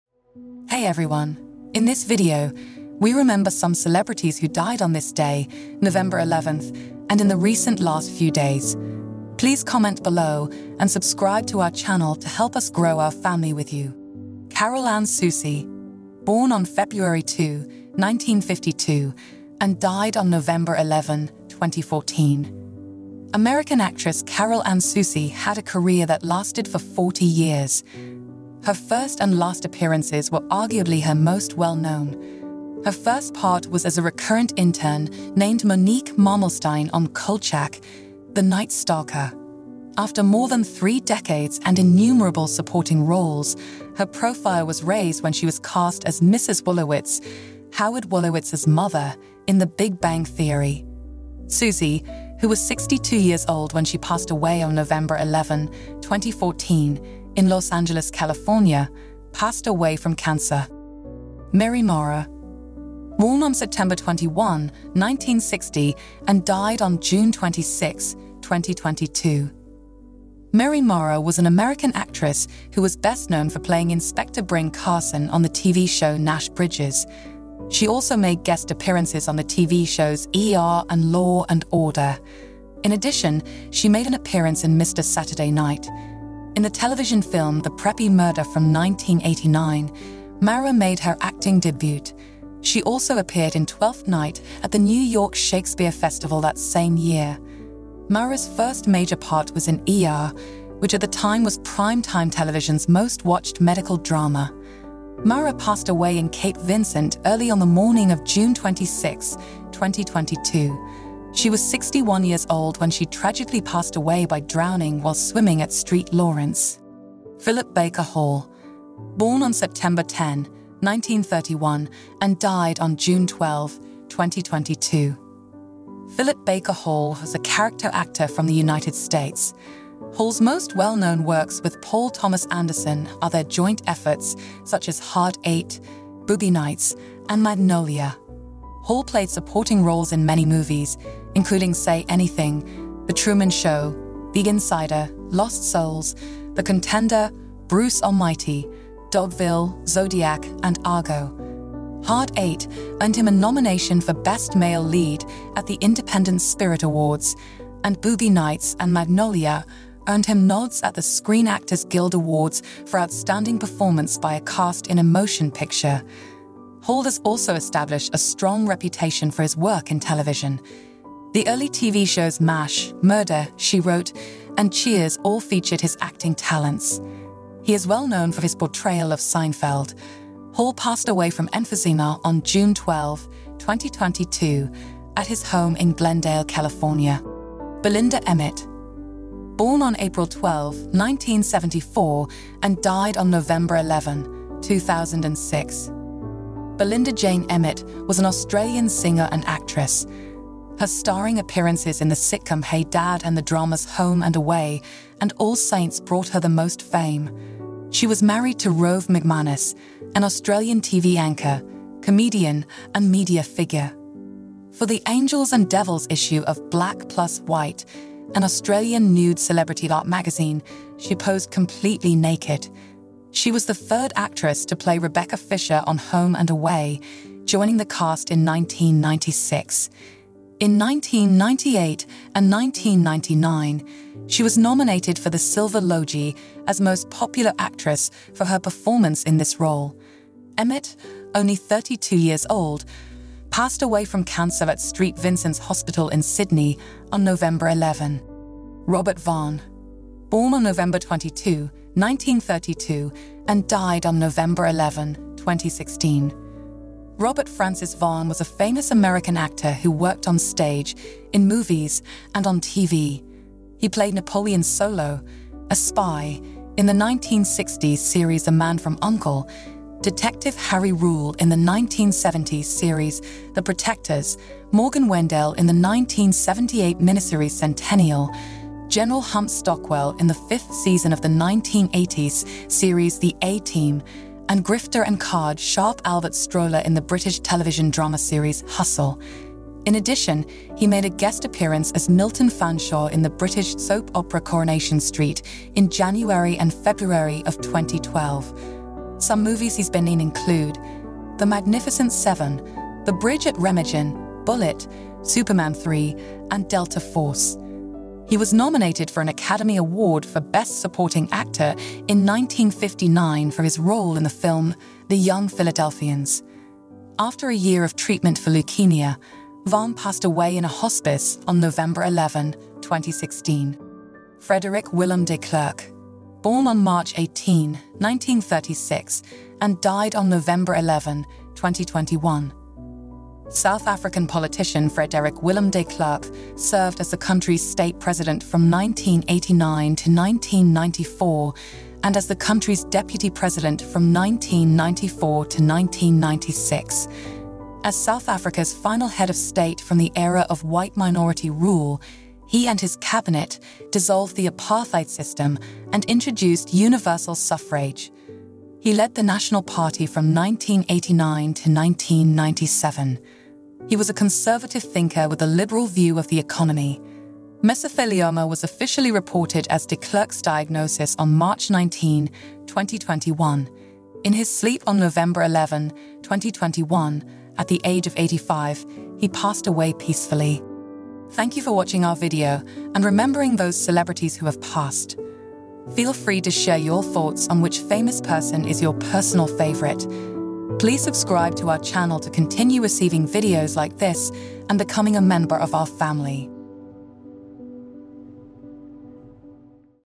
Famous Actresses And Celebrities Who Died Today (11th November), And Recent Days In 2022
This video will uncover the deaths of big actors, actresses, athletes, and celebrities who have died today (on this day), on 11th November, and over the last few days in 2022. We'll give you an overview of their lives and some of their most famous work. This list includes famous people like Carol Ann Susi, Mary Mara, Philip Baker Hall, Belinda Emmett, Robert Vaughn, Frederik
Willem de Klerk.

If you're looking for a saddening but informative video, then you've come to the right place! We'll give you a summary of the lives of these celebrities, as well as some of their most famous work. This video is a great way to learn about and remember some of your favorite celebrity's lives.

► Video footage: All video footage and images used are licensed through either CC-BY or from various stock footage websites.